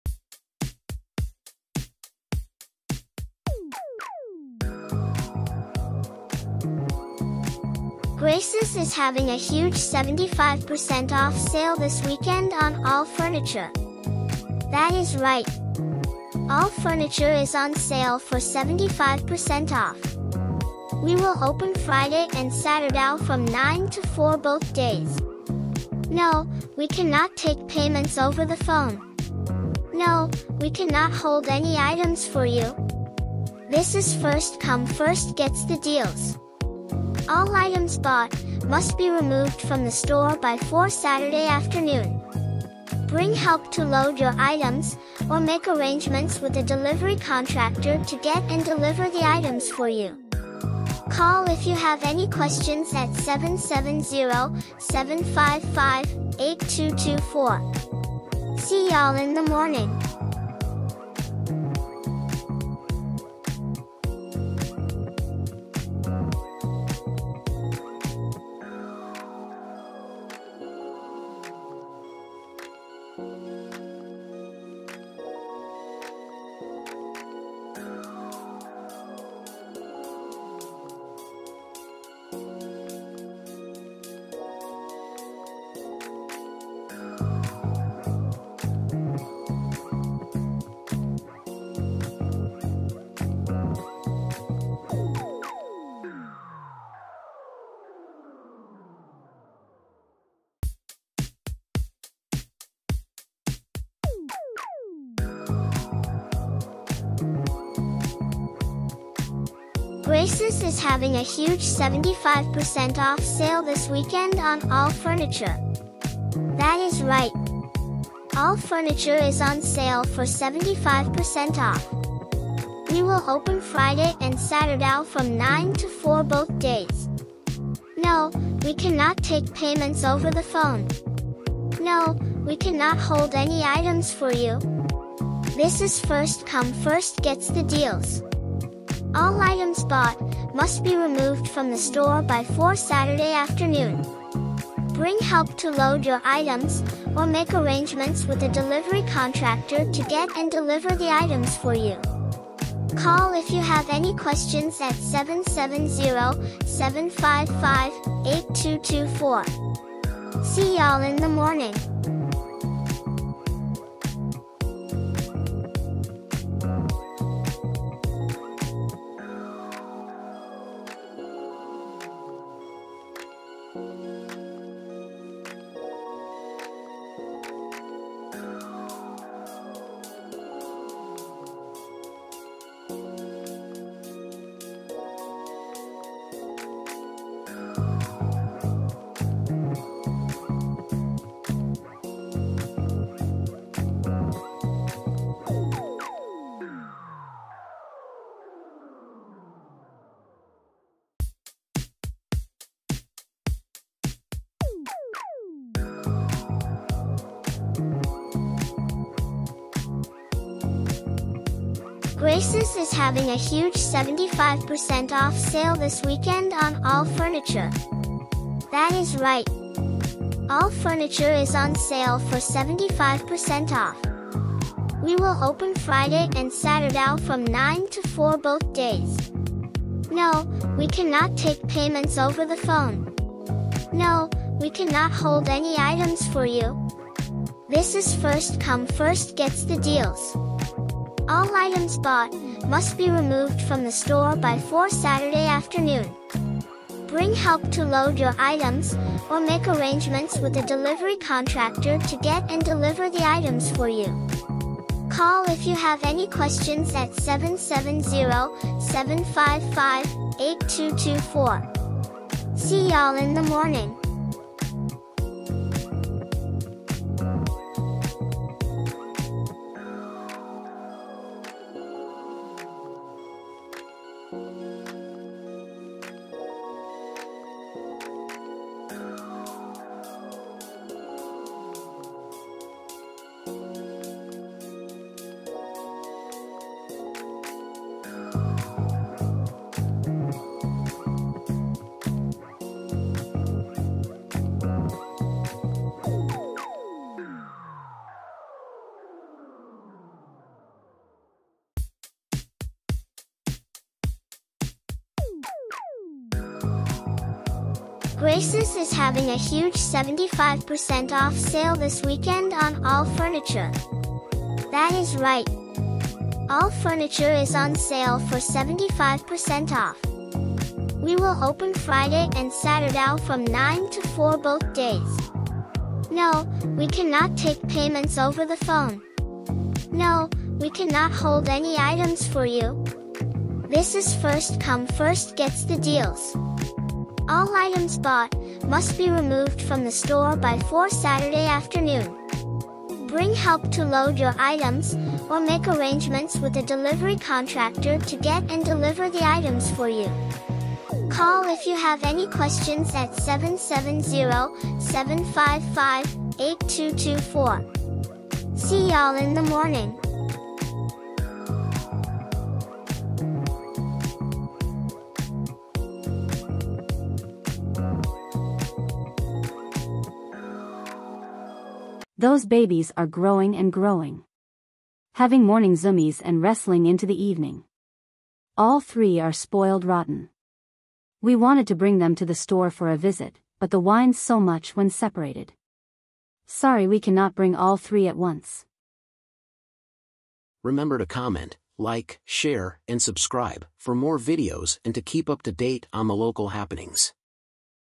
Graces Treasure Chest Thrift Store 75% Off All Furniture Sale Friday and Saturday Franklin GA
Huge discount on our online items.

Thousands of $1.00 items on sale this weekend. Furniture, electronics, collectables and jewelry all priced to move. Remember all used clothes, books, and pre-owned shoes are free at Grace's 12740 GA 34 Franklin, GA 30217
 Right off the square in downtown Franklin. Next to the Auction House behind Bank OZK. Open Fridays and Saturdays from 9-4 eastern time.